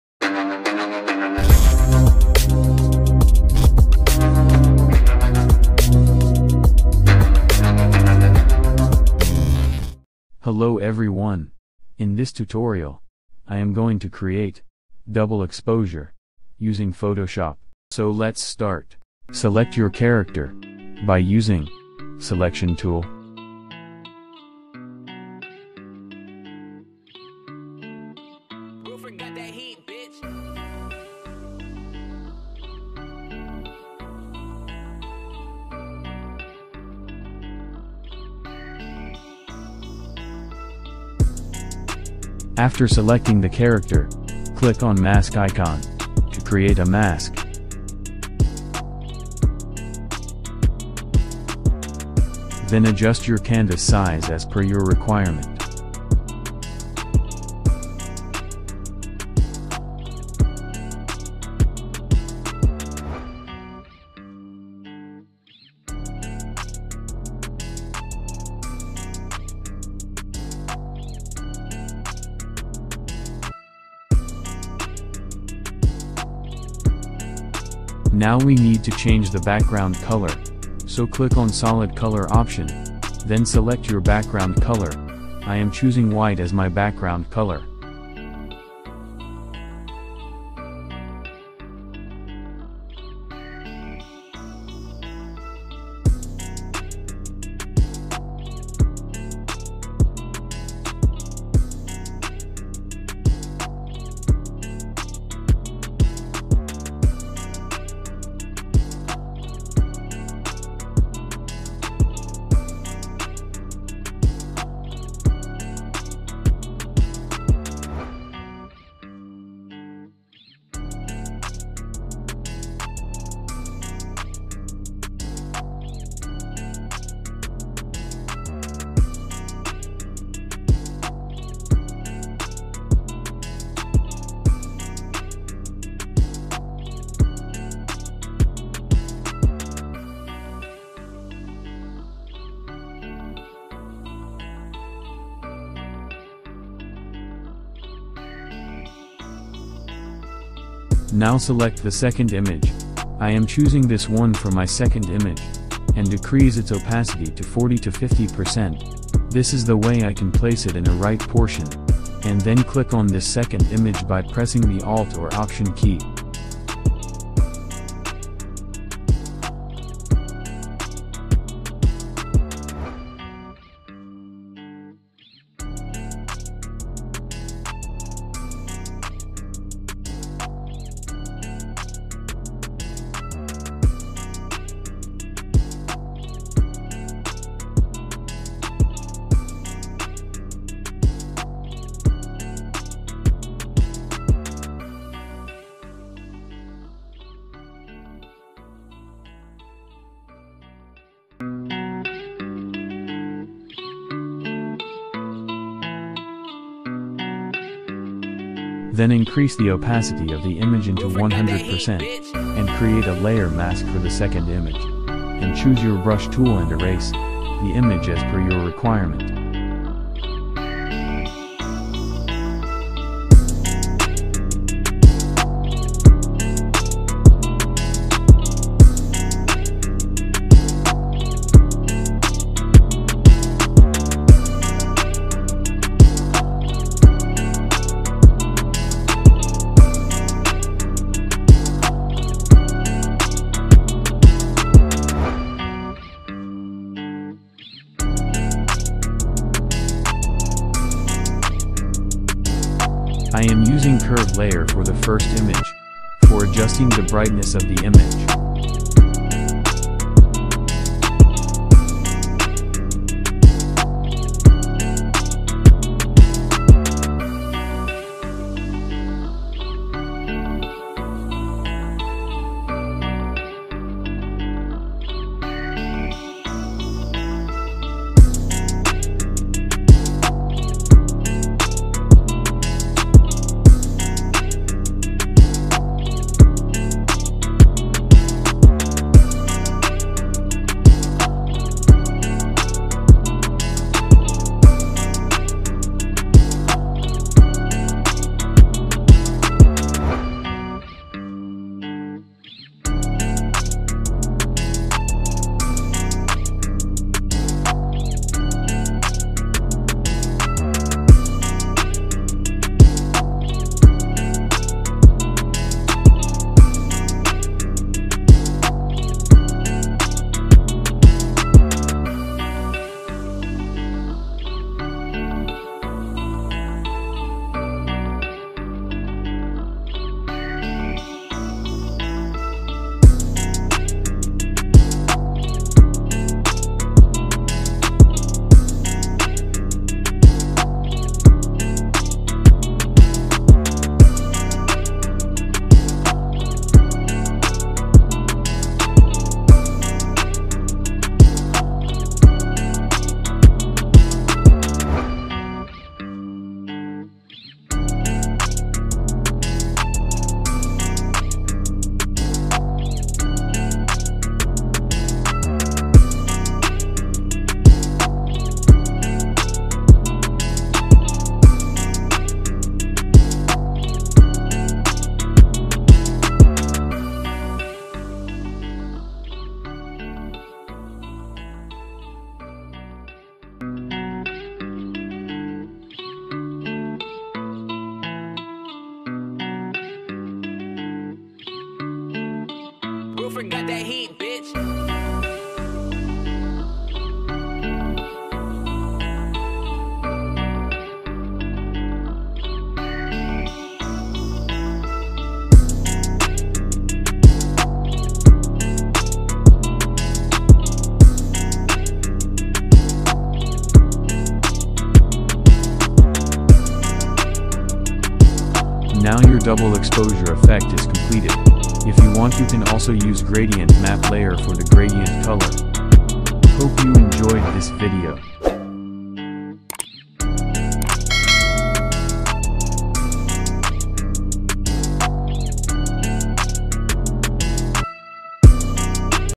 Double Exposure Effect in Photoshop | Easy Tutorial | Lion Double Exposure
In this tutorial I am going to Create double exposure Using adobe photoshop cc 2019. I am using two different images for creating this.